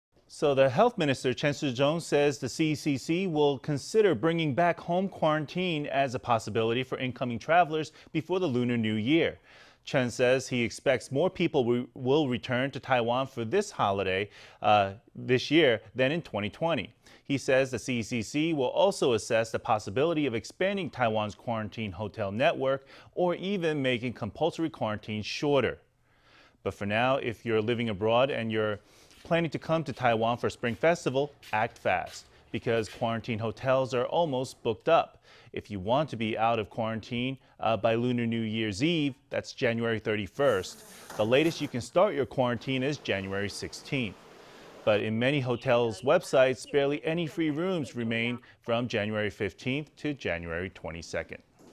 CECC to assess whether to reopen home quarantine ahead of Lunar New Year
Health minister Chen Shih-chung says the CECC will consider bringing back home quarantine as a possibility for incoming travelers before the Lunar New Year. Chen says he expects more people will return to Taiwan for the holidays this year than in 2020. He says the CECC will also assess the possibility of expanding Taiwan’s quarantine hotel network or even making compulsory quarantines shorter.

But for now, if you’re living abroad and you’re planning to come to Taiwan for Spring Festival, act fast, because quarantine hotels are almost booked up. If you want to be out of quarantine by Lunar New Year’s Eve, on Jan. 31, the latest you can start your quarantine is Jan. 16. But in many hotels websites, barely any free rooms remain from Jan. 15 to Jan 22.